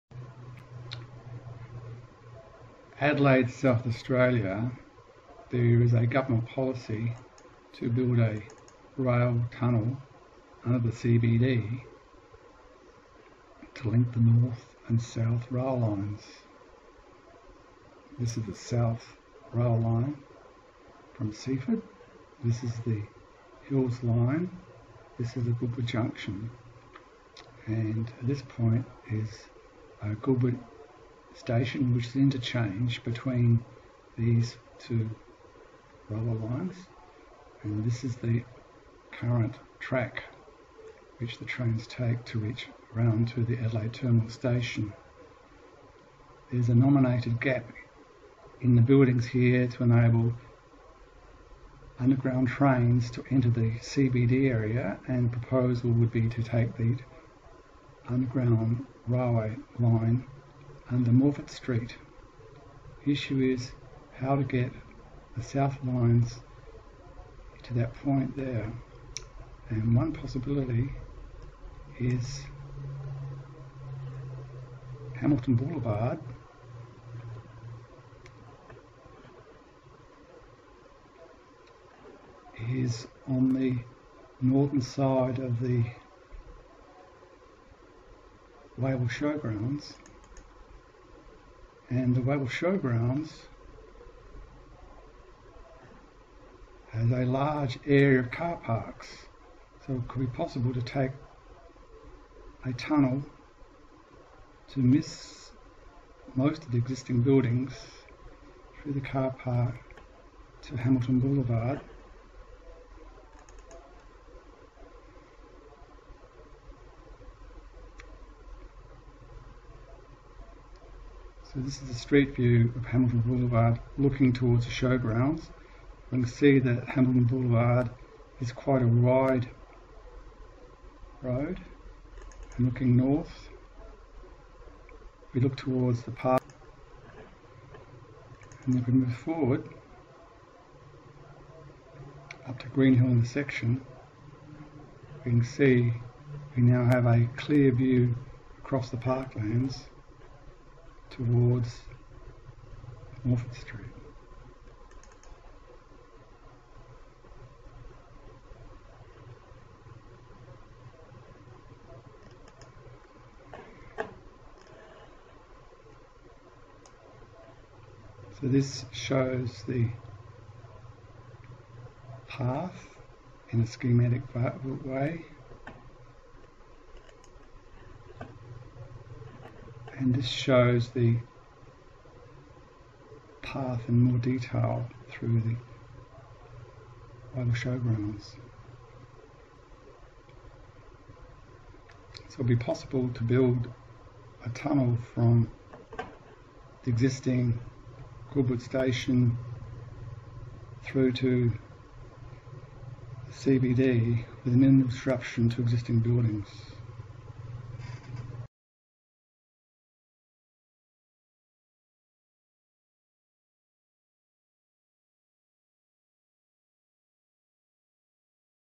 North South Rail Tunnel via Hamilton Bvd Adelaide April 2019 video
North South Rail Tunnel could gain access to the Seaford Rail Line via a tunnel under the Wayville Showgrounds.     The tunnel would continue under Hamilton Bvd and the under the Park Lands to a tunnel under Morphett St.    The tunnel would use the gap in the buildings on North Terrace to have a portal in the "Nairne Junction" section of the Adelaide Terminal Rail Station